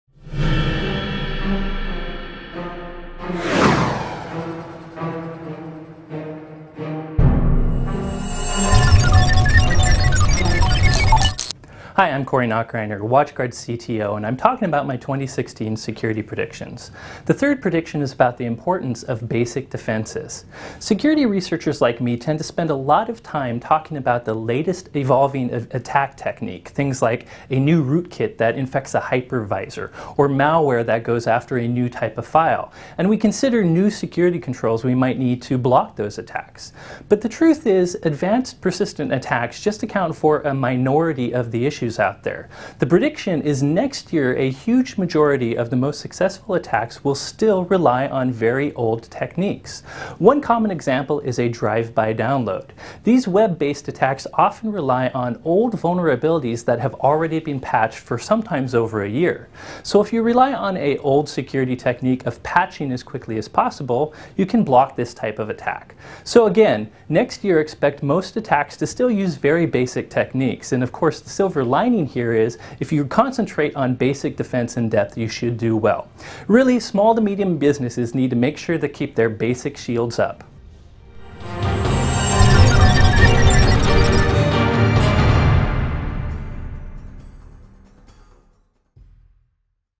2016 Cybersecurity Predictions - #3: SMBs Can’t Let Basic Shields Down
Security Breaches Go Back to Basics: A majority of successful security attacks –especially ones against smaller targets– still rely on the basics. Despite some threat actors using sophisticated techniques, most Small to-Medium Business (SMB) security breaches will come back to basic security best practice failures. There is a silver-lining. If you concentrate on following basic security best practices, you will avoid a majority of the attacks in 2016.